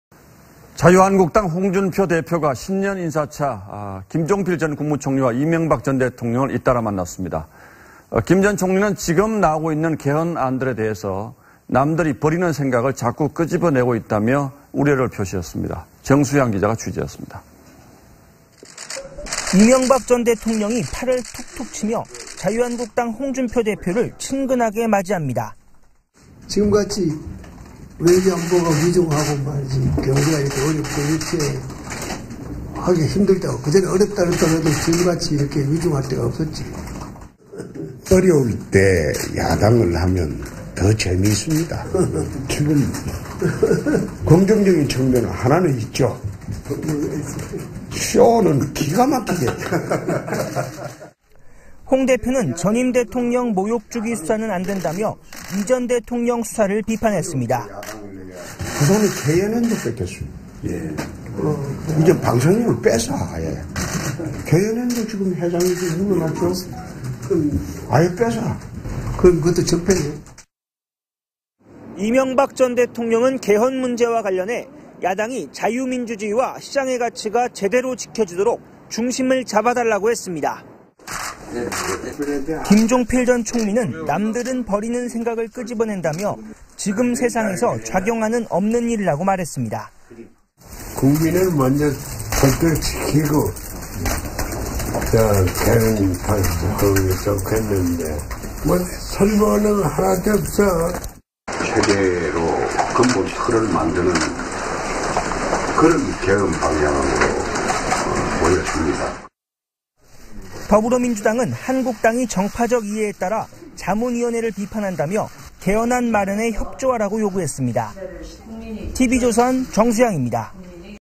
MB·JP 개헌안 비판 "남들은 버리는 생각 끄집어내"
자유한국당 홍준표 대표가 신년 인사 차 잇따라 김종필 전 국무총리와 이명박 전 대통령을 만났습니다. 김 전 총리는 지금 나오고 있는 개헌안에 대해 "남들이 버리는 생각을 자꾸 끄집어 내고 있다며 우려를 표시했습니다.